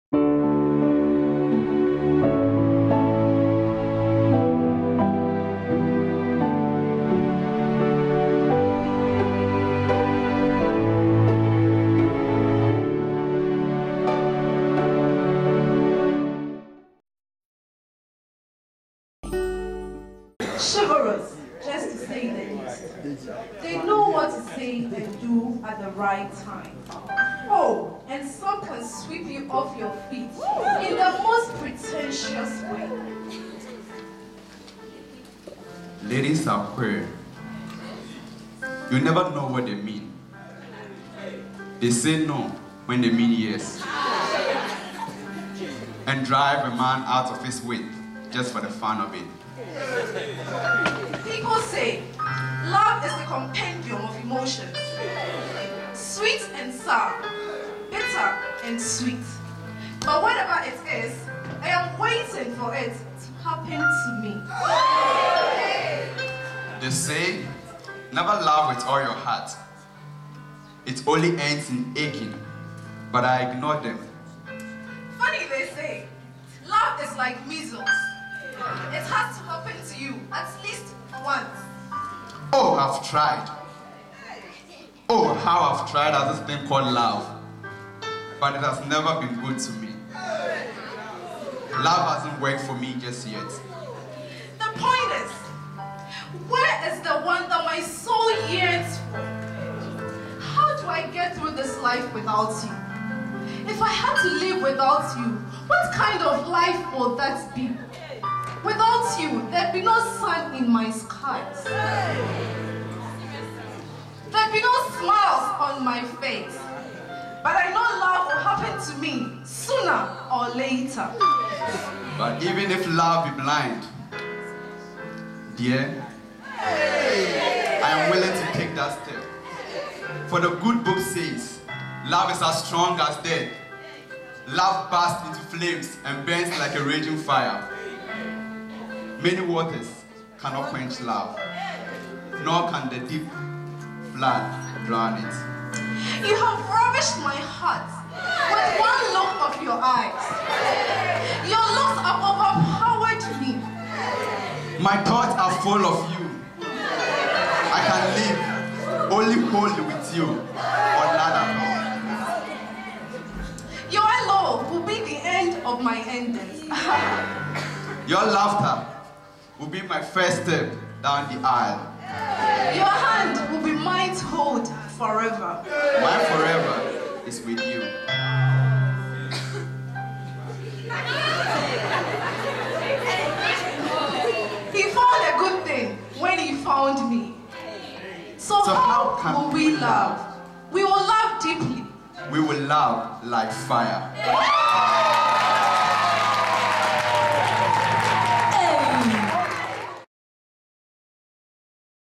Special Valentine Presentation by Charles and Josephine
These valentine poem was read during the Valentine service in ICA Moscow by Josephine Quarcoo and Charles Debra